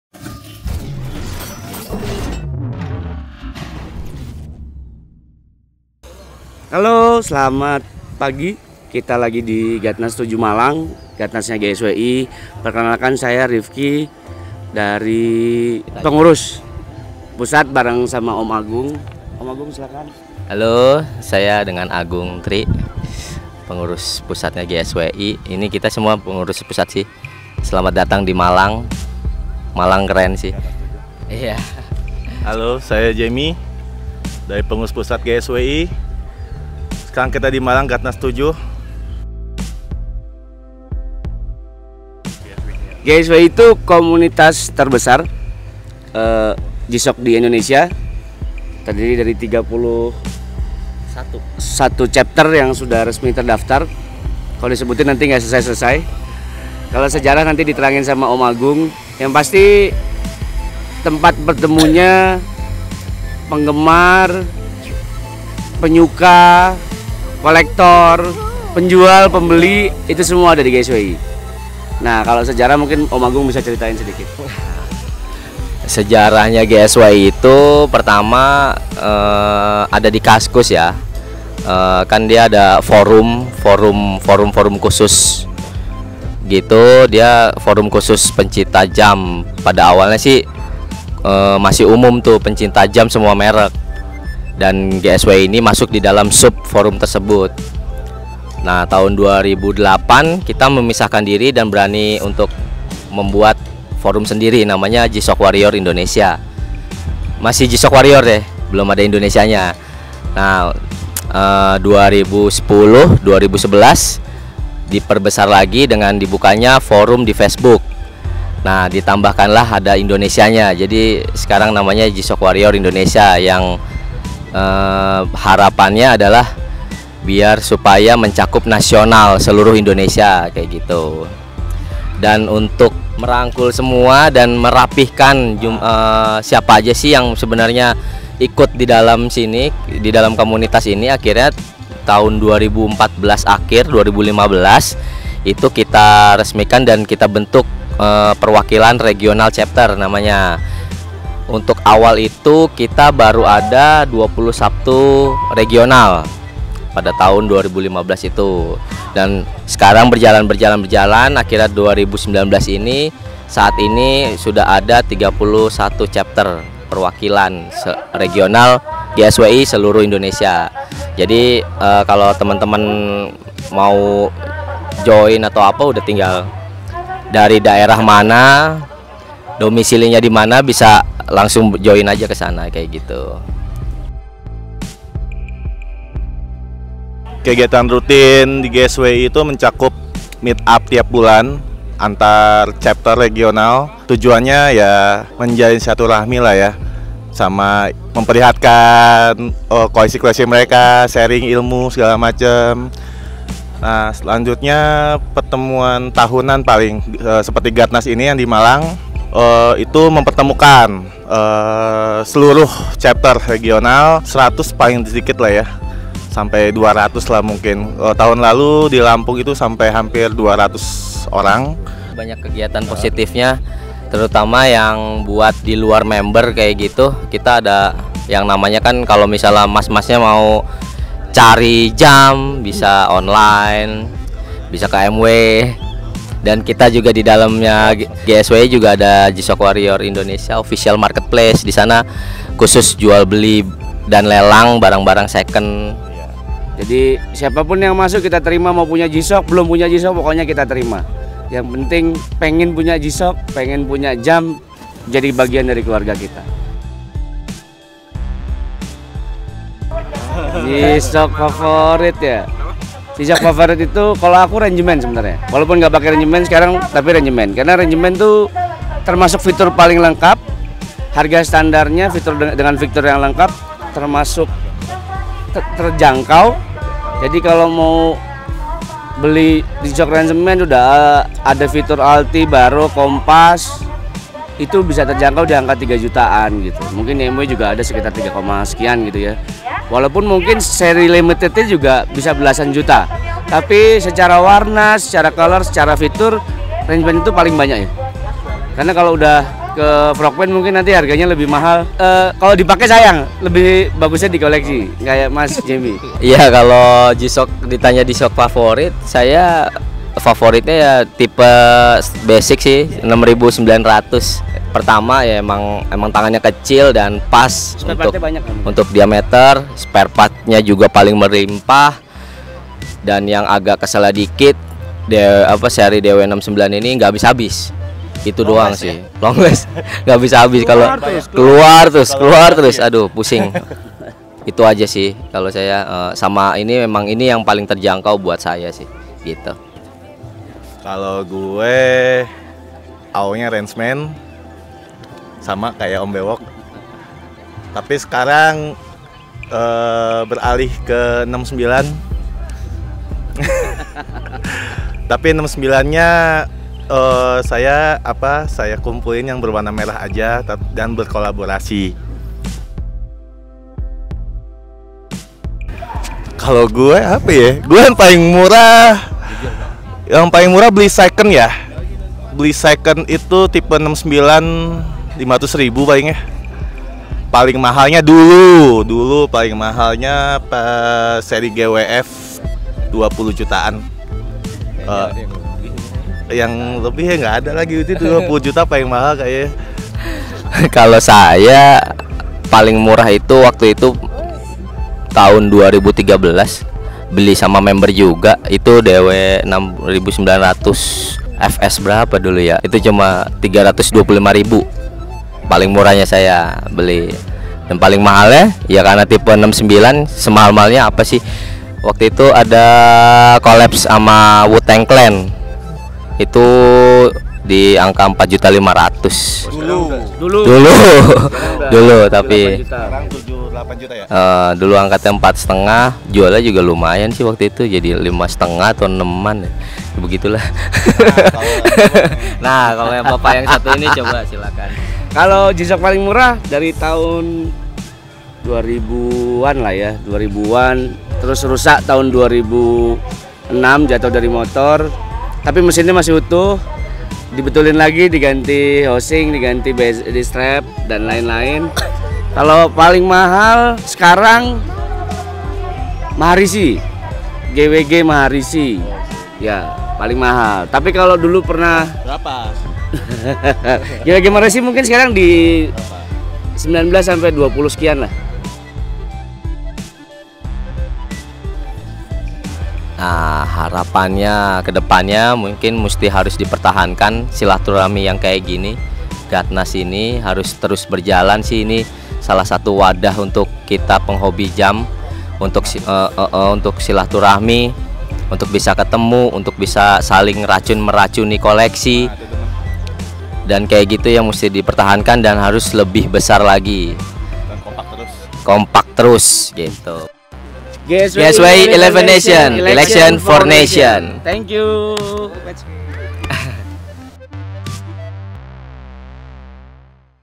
Mengenal GSWI (G-Shock Warriors Indonesia) Lebih Dekat
G-SHOCK WARRIOR INDONESIA
Pecinta G-SHOCK pasti sudah tidak asing dengan GSWI atau kamu bagian dari GSWI? Naah buat kamu yang ingin memiliki G-SHOCK ada keluarga yang akan menerimamu. GSWI merupakan kumpulan pecinta, kolektor, penjual, dan pembeli G-SHOCK jam tangan dari Jepang yang sudah tidak diragukan lagi durabilitasnya. Banyak sekali informasi yang bisa kamu dapatkan mengenai jam tangan kesayangmu disini. Ingin memahami lebih jelas tentang G-SHOCK? tonton video ini tanpa diskip yaa!!